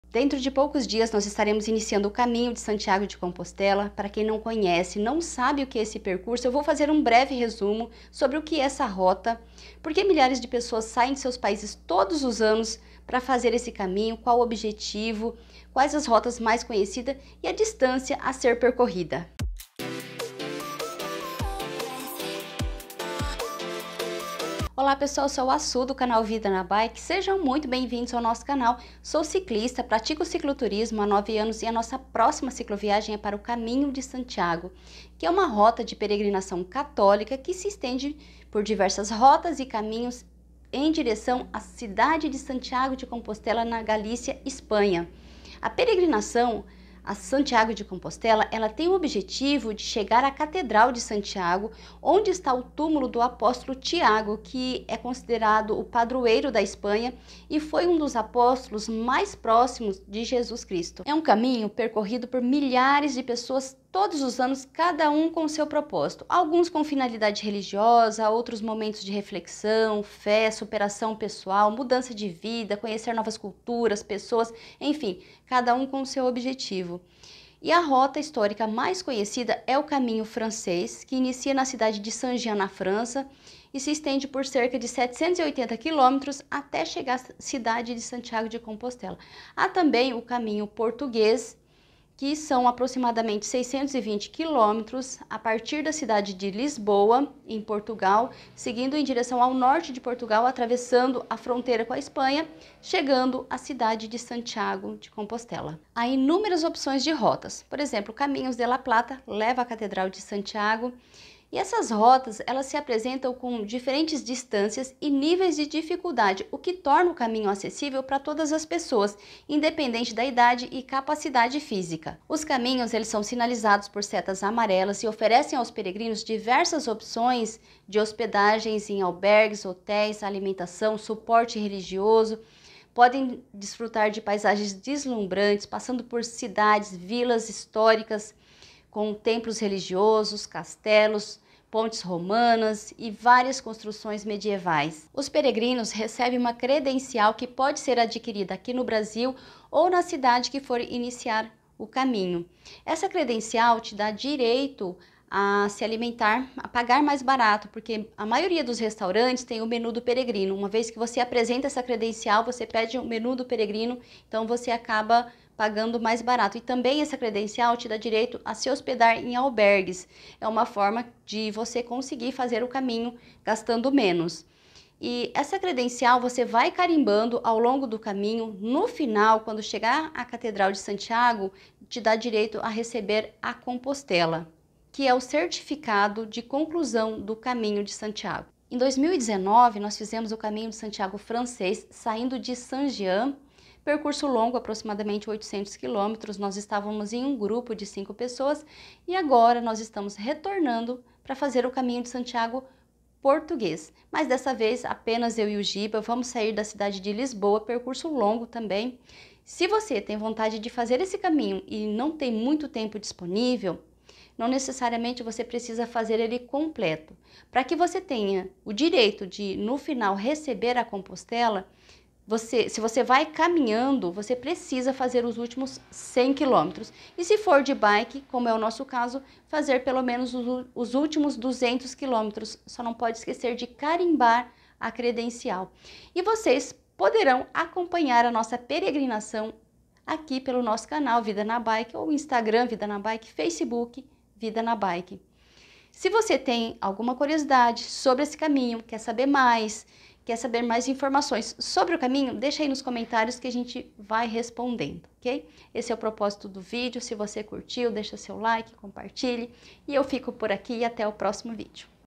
CAMINHO DE SANTIAGO DE COMPOSTELA 2023
Caminho de Santiago é uma rota de peregrinação católica.

Existem diversas rotas a mais conhecida é o caminho Francês que sai da cidade de SAN Jean na França e se estende por cerca de 780 km em direção à catedral de Santiago onde está o túmulo do apóstolo Tiago que é o padroeiro da Espanha e foi um dos apóstolo mais próximo de Jesus!

Há também o caminho português que se inicia da cidade de Lisboa em Portugal, seguindo pelo norte de Portugal, são aproximadamente 620 km até chegar a cidade de Santiago de Compostela.

Os caminhos são sinalizados com setas amarelas e oferecem aos peregrinos diversas estruturas de hospedagem, albergues hotéis e pode desfrutar de belas paisagens, templos religiosos, construções medievais!

Todos os anos milhares de pessoas realizam essa peregrinação, seja caminhando, de bike ou a cavalo.
Cada um com seu propósito, alguns com finalidades religiosa, superação pessoal, momentos de reflexão, fé…

O caminho tem o poder de nos desafiar e nos transformar a cada quilômetro percorrido, é uma experiência única e intensa que vale a pena ser vivida!